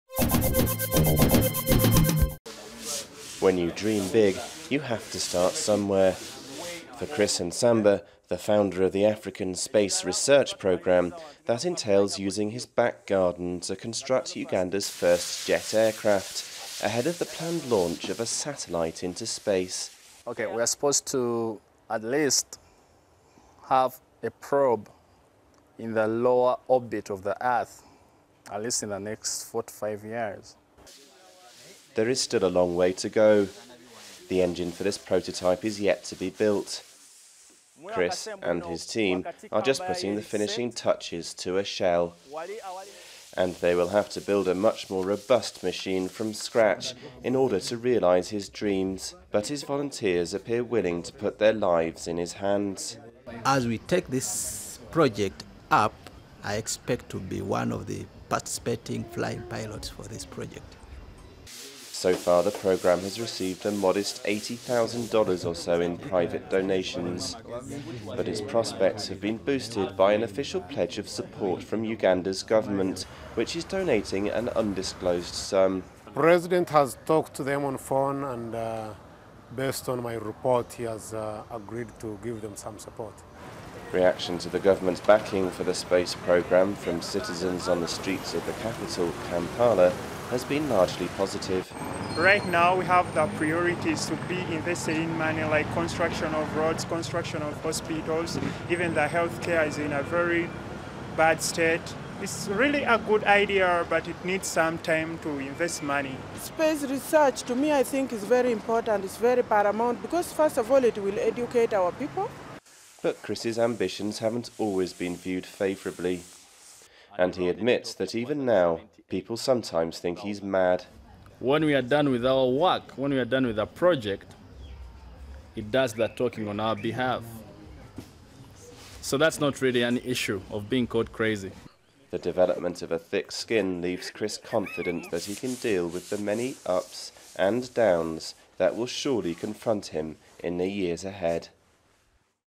Uganda launches bid to lead Africa into space
Uganda may not be the first nation that springs to mind in the field of space exploration -- but the impoverished central African country is setting its sights on becoming a regional research hub. Duration: 02:22